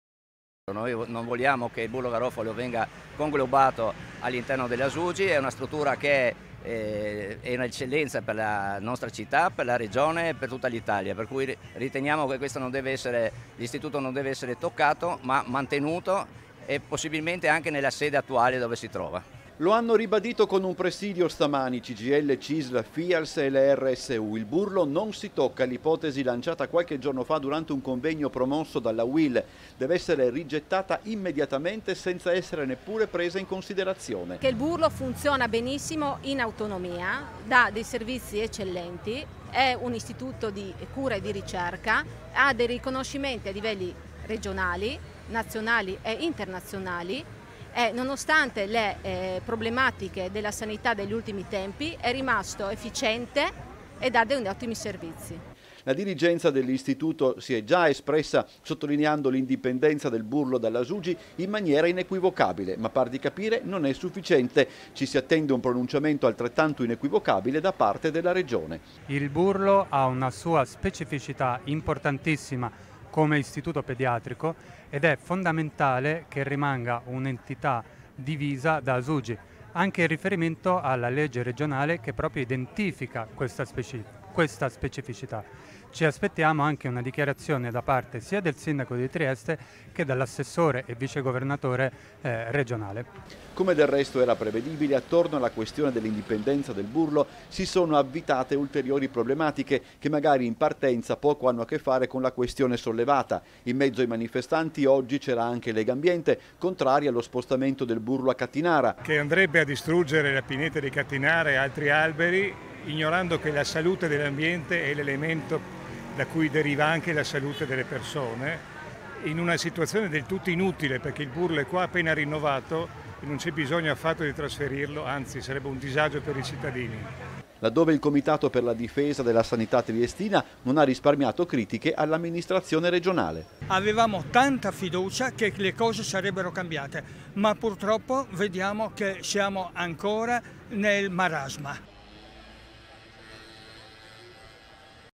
'IL BURLO RESTI INDIPENDENTE': VA IN SCENA LA PROTESTA | 17/06/2022
TG sera 17/06/2022 - TRIESTE - Presidio, stamani davanti al Burlo Garofolo, per ribadire l'indipendenza dell'Istituto da Asugi. Si attende un pronunciamento, netto, dalla Regione. - Intervistati FABIO POTOTSCHNIG (segretario regionale FIALS), FRANCESCA FRATIANNI (CGIL Funzione Pubblica), fulvio CELSI (CISL), paolo angiolini (direttivo Legambiente), BRUNO BALDAS (COMITATO DIFESA SANITA' TRIESTINA) (Servizio di Umberto Bosazzi) - Segui Telequattro anche sul digitale terrestre! Visita il sito